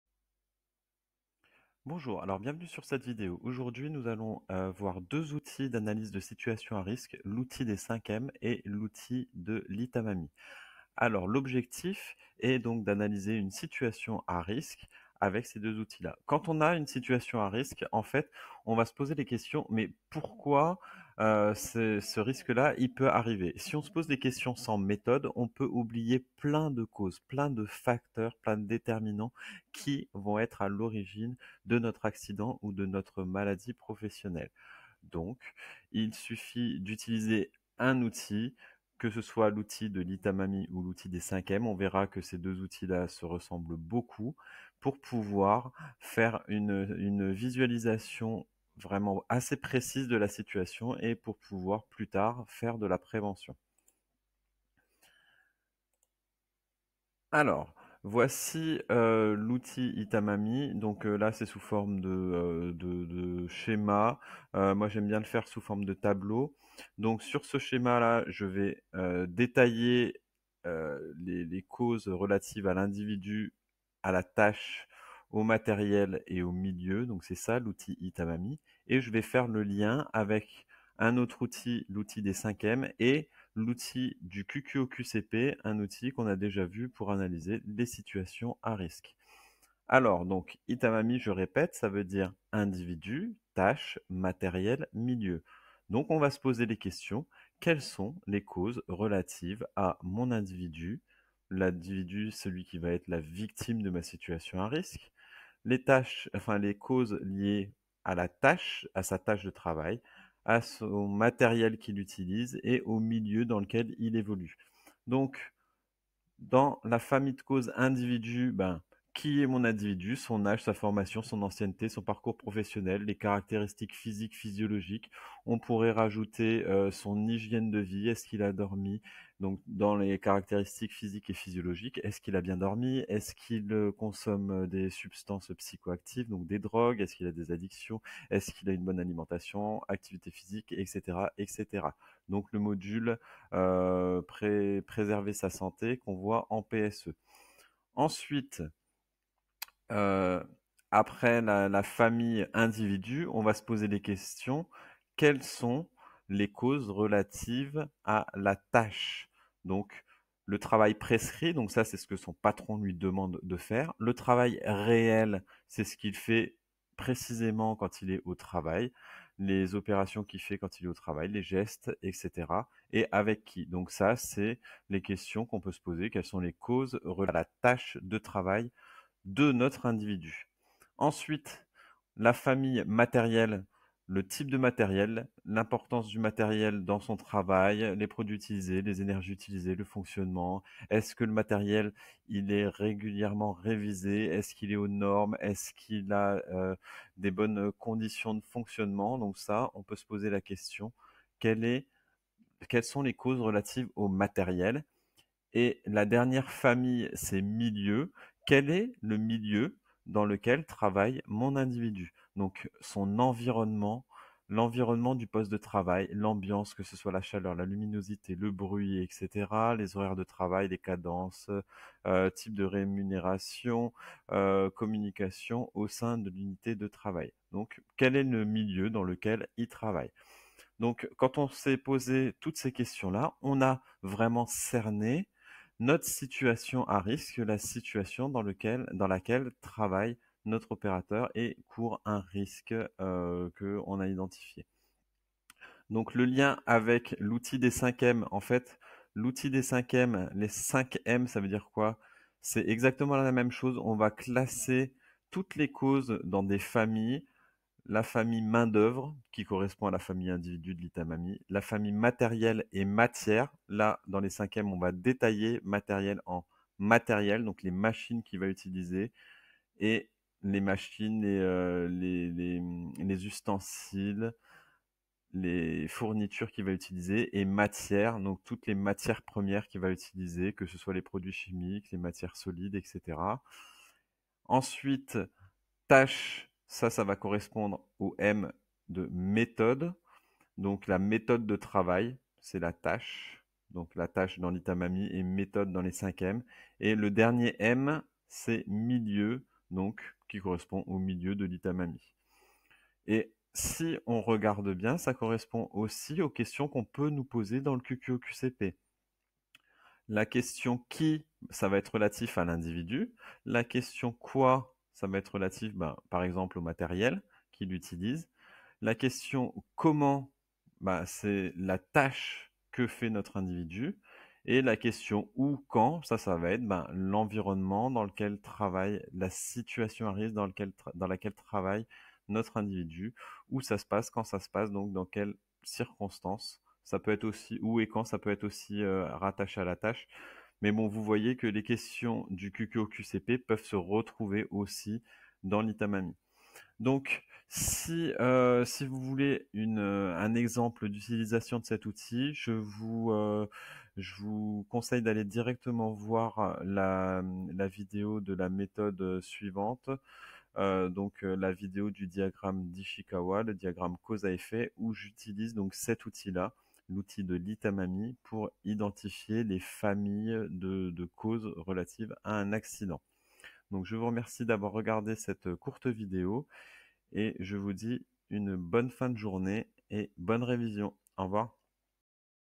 Outil 02 - ITaMaMi + 5M
Niveau :     Débutant ( CAP1 ; 2nde BacPro)
                   Confirmé ( CAP2 ; 1ère BacPro)
                   Expert      ( Terminale Bac Pro)

Démarche de prévention :
           1  -  Identifier les éléments pertinents
           2  -  Utiliser des outils d'analyse
           3  -  Définir la problématique
           4  -  Evaluer le risque
           5  -  Identifier les actions de préventions

Erratum:
R.A.S. pour le moment